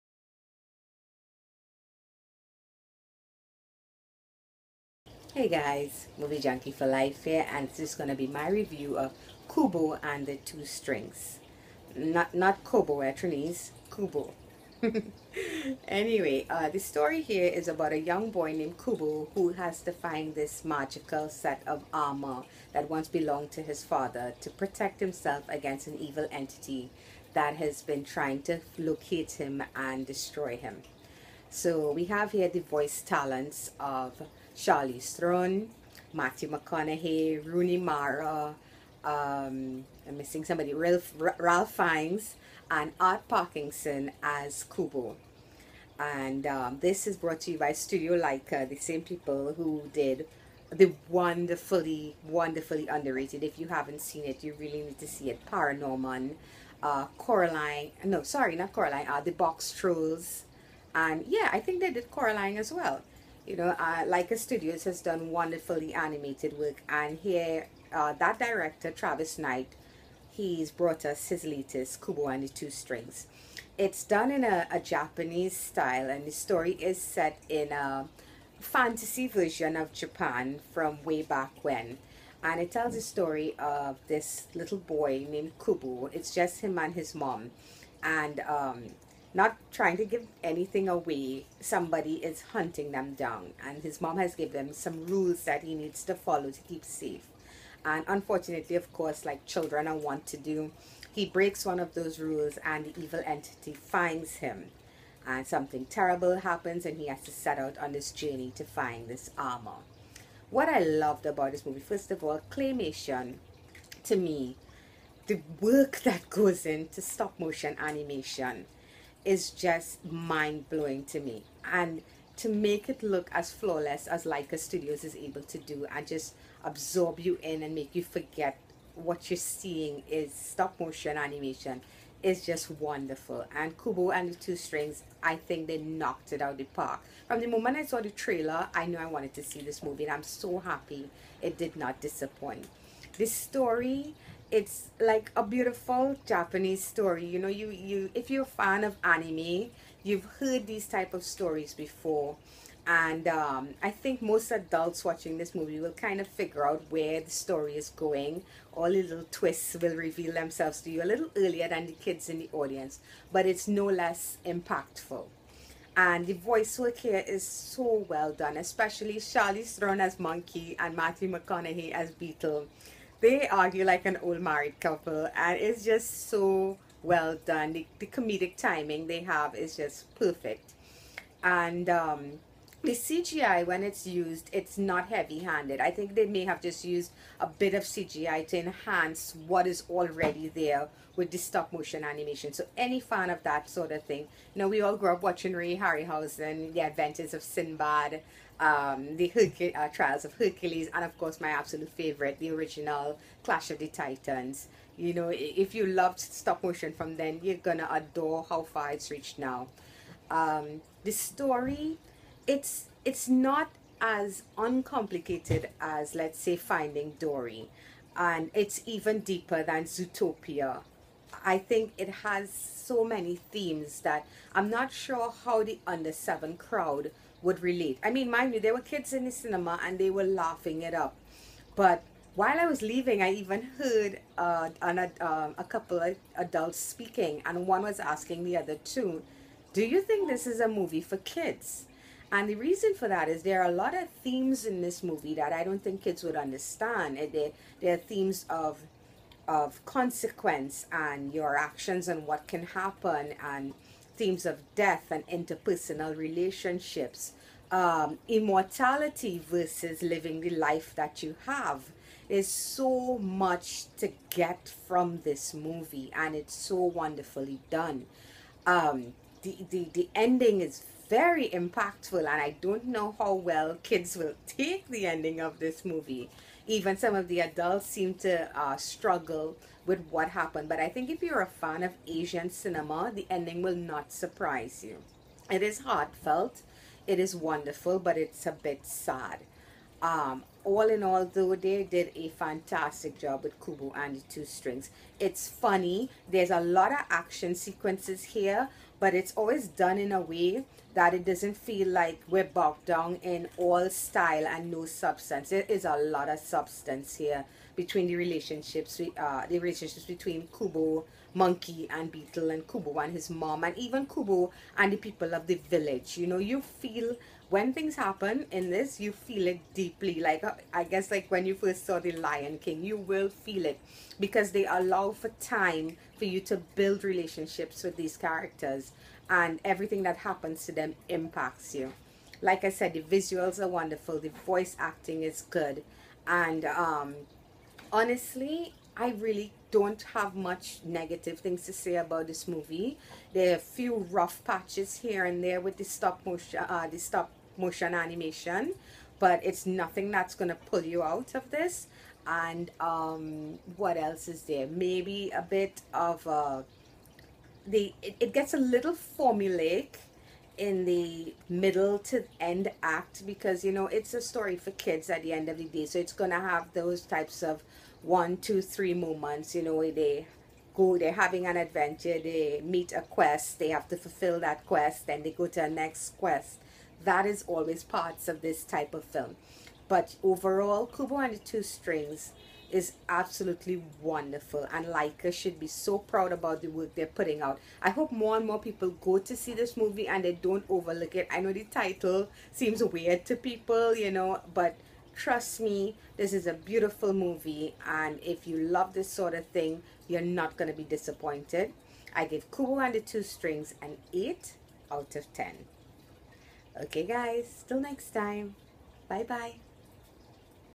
REVIEW KUBO AND THE 2 STRINGS
While CG has given us memorable stories and visuals, there is something about Stop Motion Animation, that wows me every single time. Here's my review of KUBO & THE TWO STRINGS.